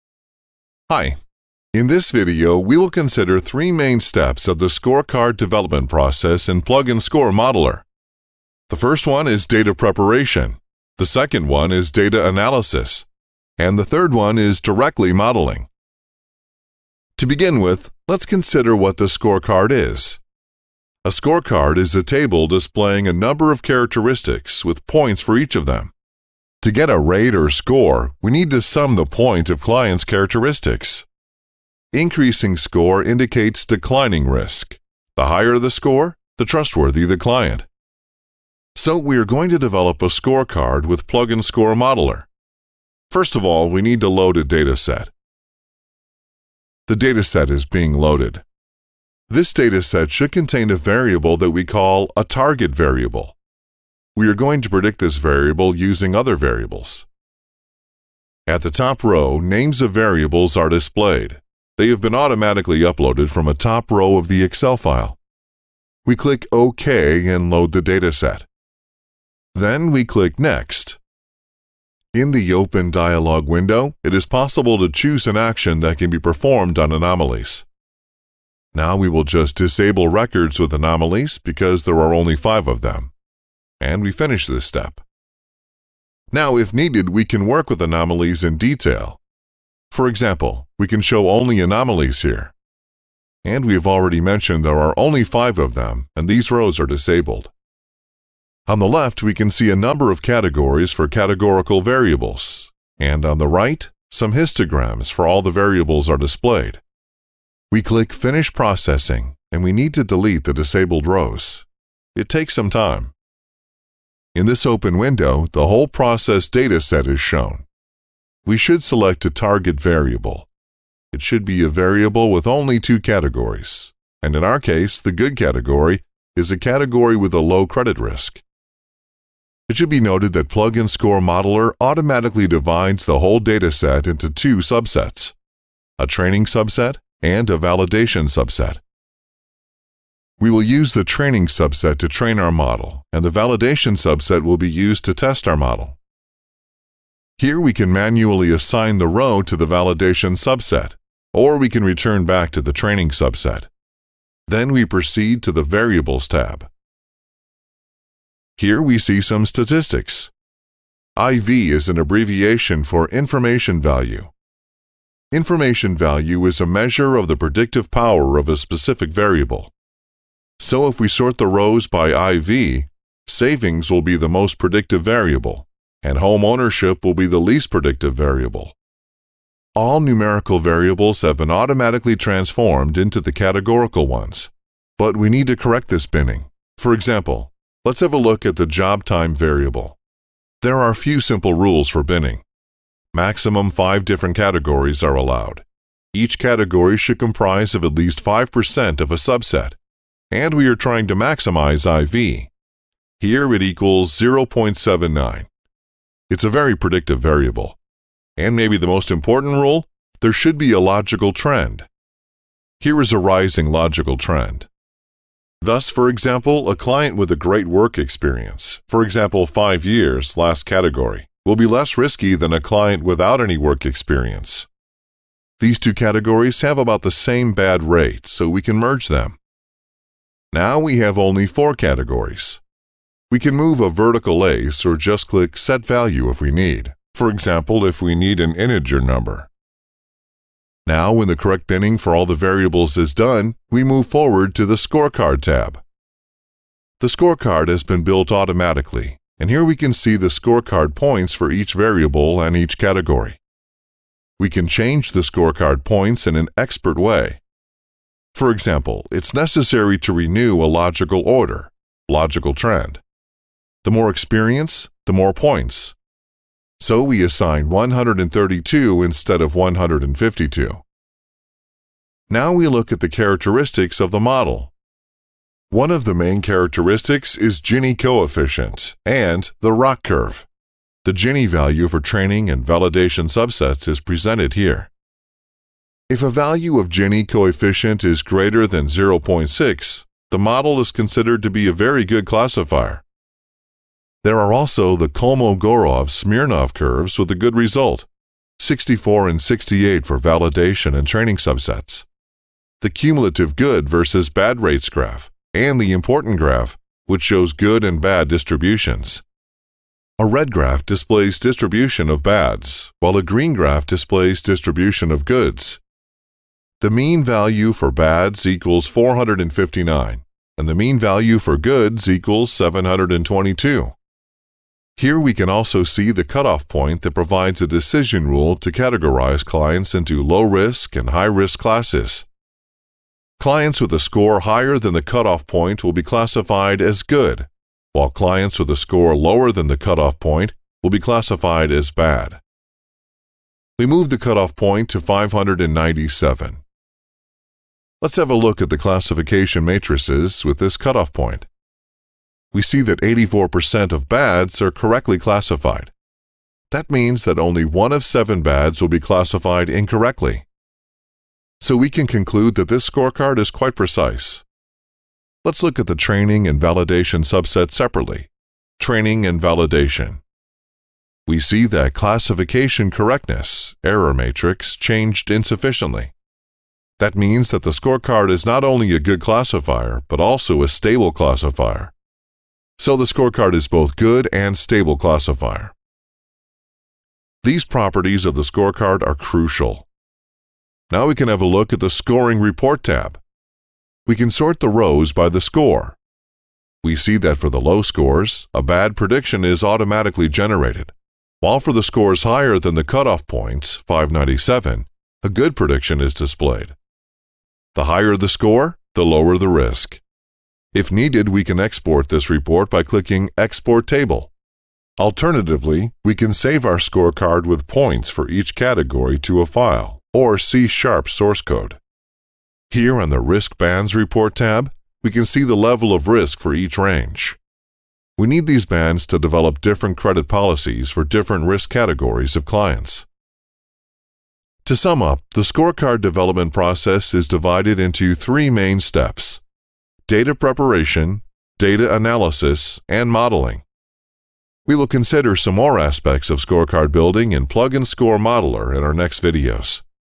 How to Create a Scorecard?
Do you know how to create a scorecard? In this movie we will consider application of variables in the scorecard development process in Plug-n-Score Modeler.

We will talk about Logistic Regression Model in our next videos.